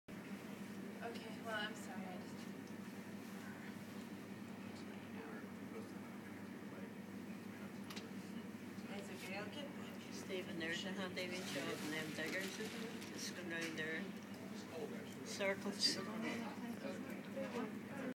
Snow Plough.MOV
Snow plough tractor in Belfast International airport 19/12/2010